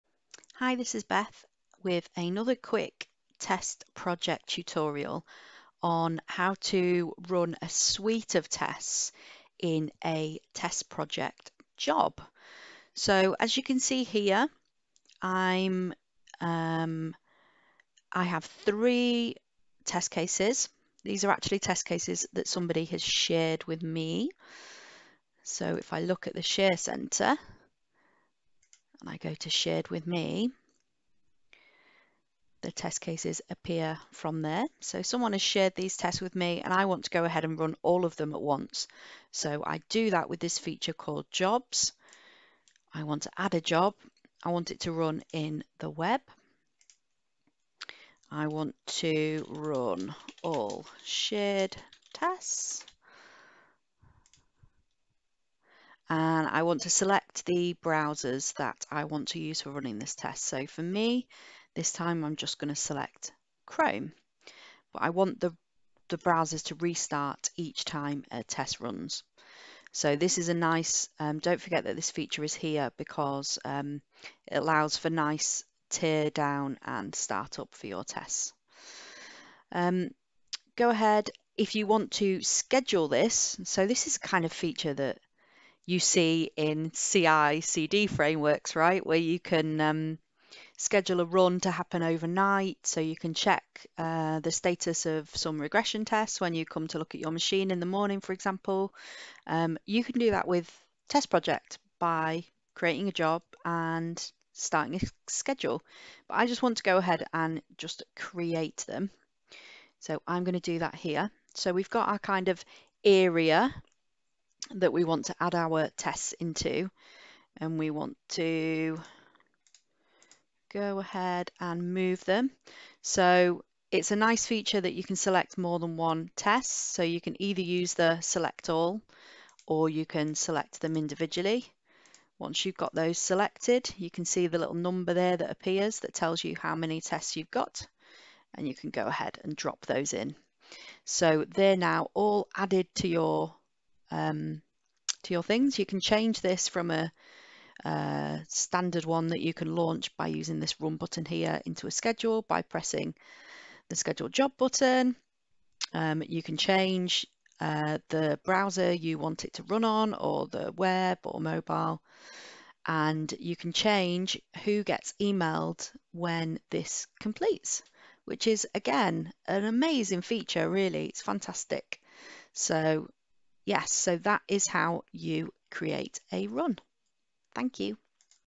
How To Create A Test Suite In Test Project
A brief video to demonstrate how you can use "runs" to create a manually triggered or scheduled set of test cases in Test Project.